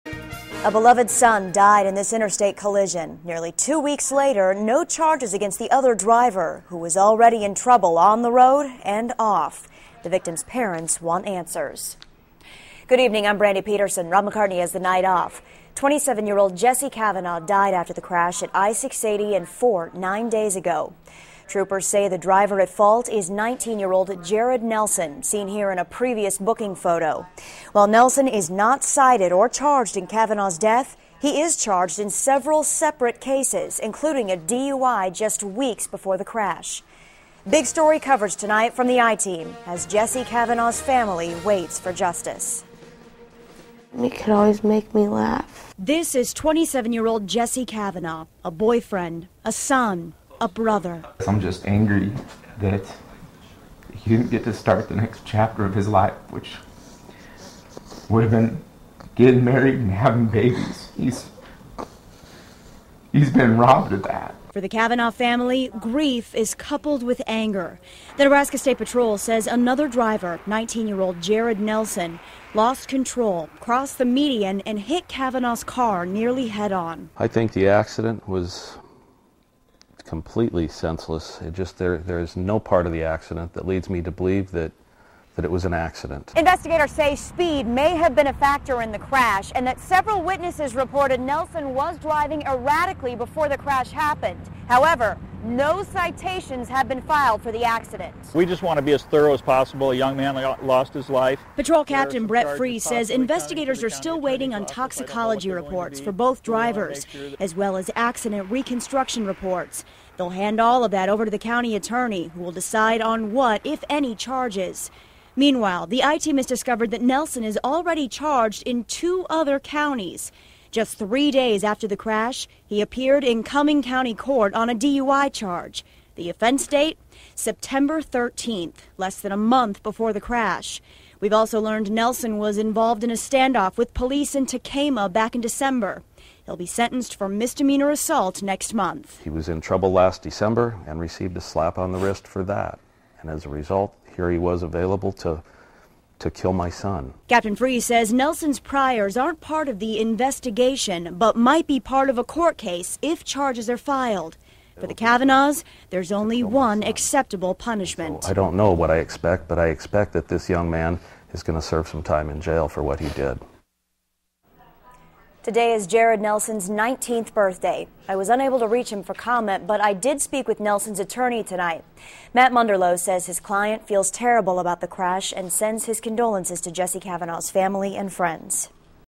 Omaha Crash Victim's Family Wants Justice
Nine days after a man died in an interstate crash in Omaha, his family wants to know why no charges have been filed against the driver responsible for the fatal collision.